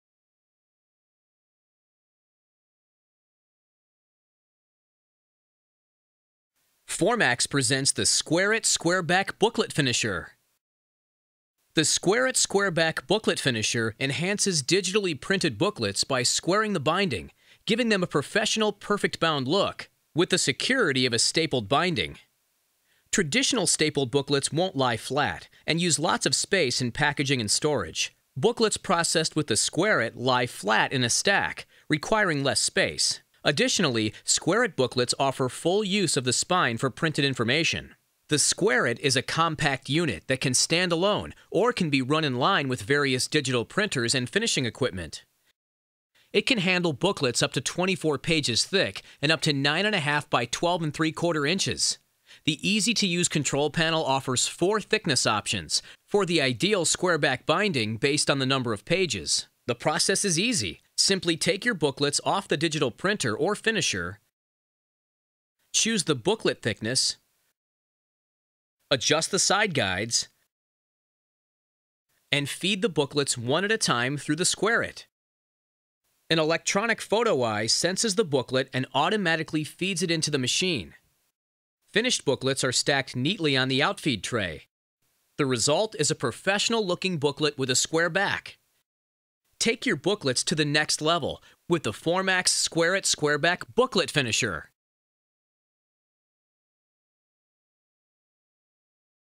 Formax Square It Squareback Booklet Finisher
Don't just fold it... Square It! The Square It Squareback Booklet Finisher from Formax gives your stapled booklets a professional appearance the ability to lie flat. Say goodbye to cracked edges and fading- a square edge is visually appealing and provides the ability to print on the spine. The Square It can finish booklets up to 24 sheets thick in sizes up to 9.5" x 12.75". An easy to use control panel allows even inexperienced operators to quickly and easily create beautiful booklets. The Square It's modular design allows it to be used as either a stand-alone unit or in-line with other finishing equipment.